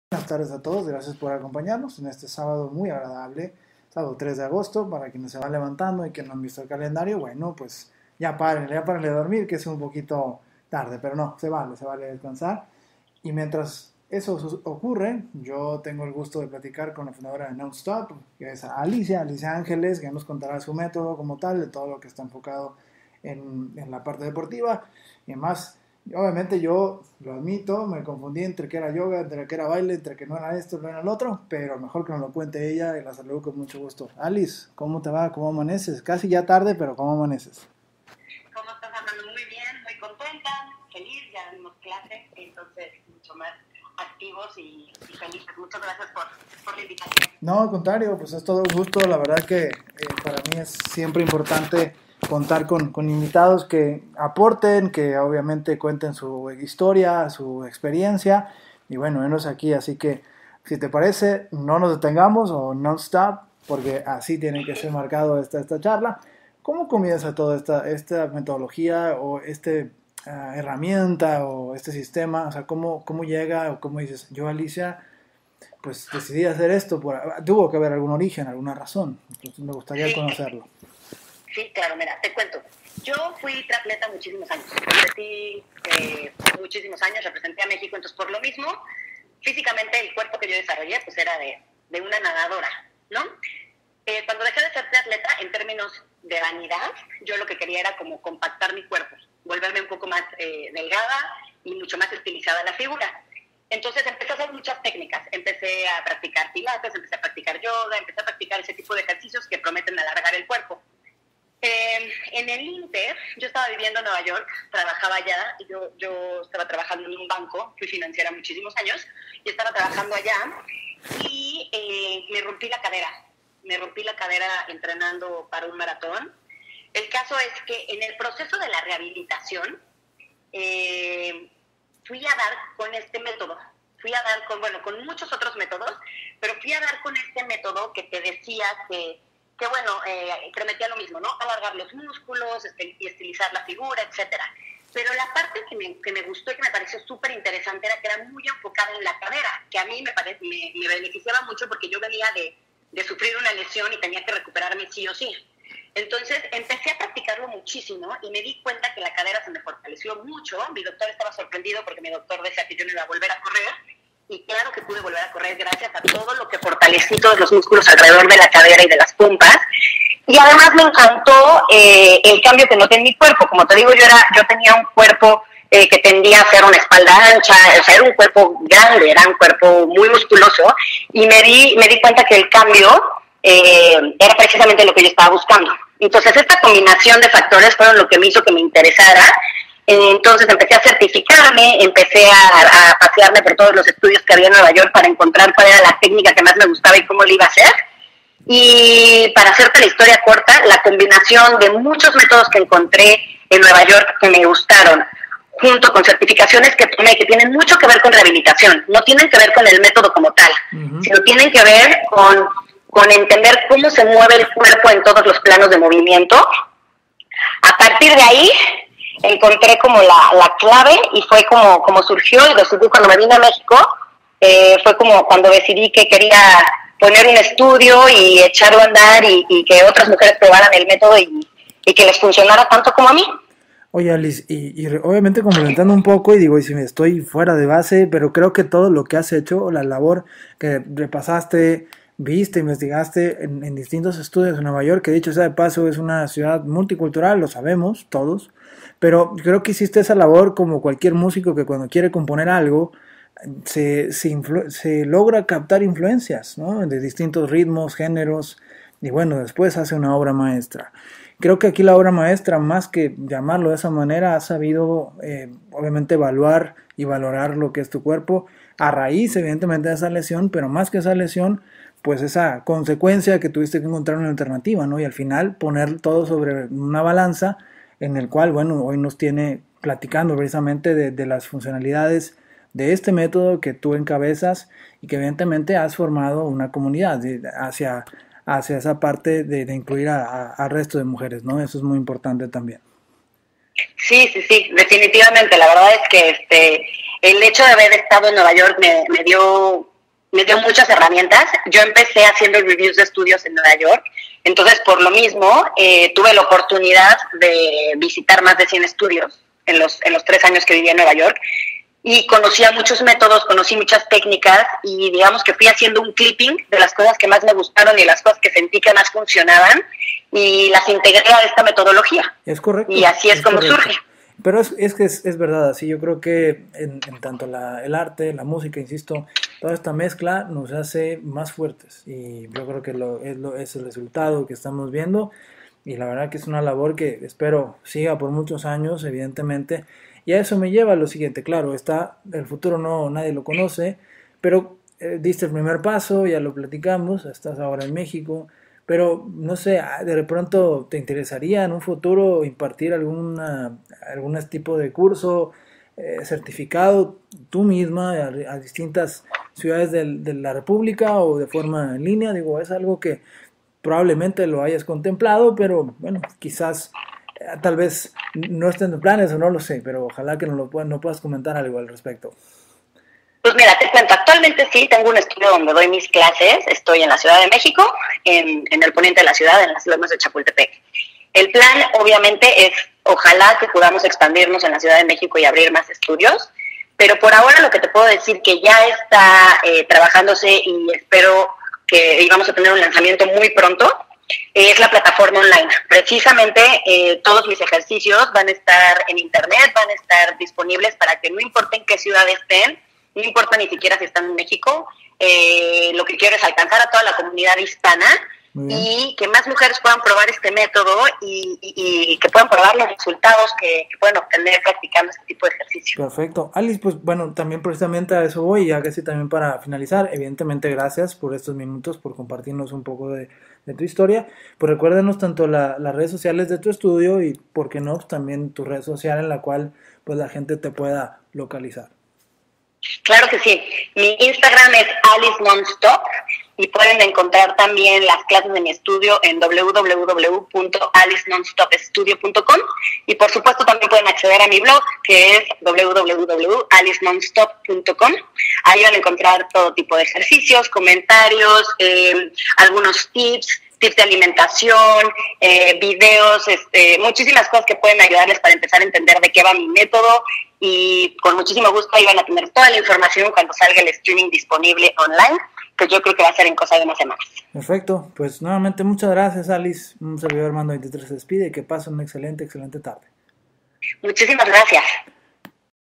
Episodio #550| Entrevista con Alice Non Stop
La combinación de métodos deportivos arroja resultados infinitamente superiores si conocemos nuestro cuerpo y en consecuencia logramos desarrollar nuestros propios métodos.